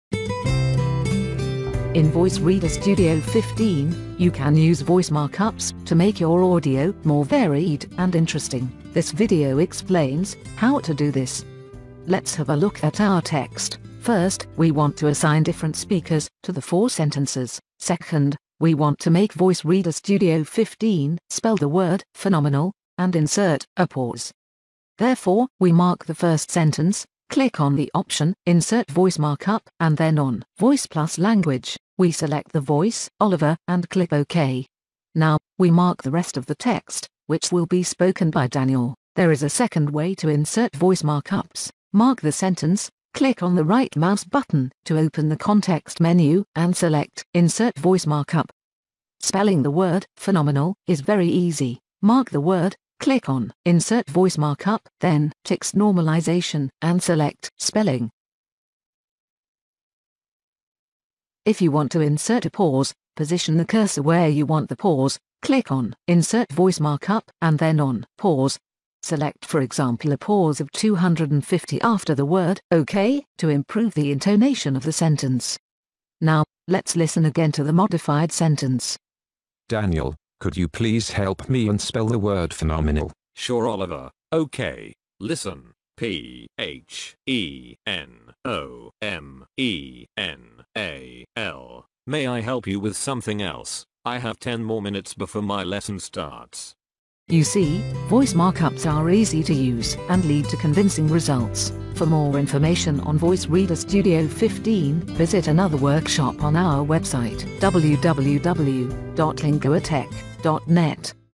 Tutorial Linguatec Text-to-Speech Voice Reader Studio 15 English - Use Voice Markups
Text-to-Speech Voice Reader Studio 15 by Linguatec converts your texts to voice professionally using natural sounding voices, which can be adjusted to suit your requirements. This tutorial shows you how to use Voice Markups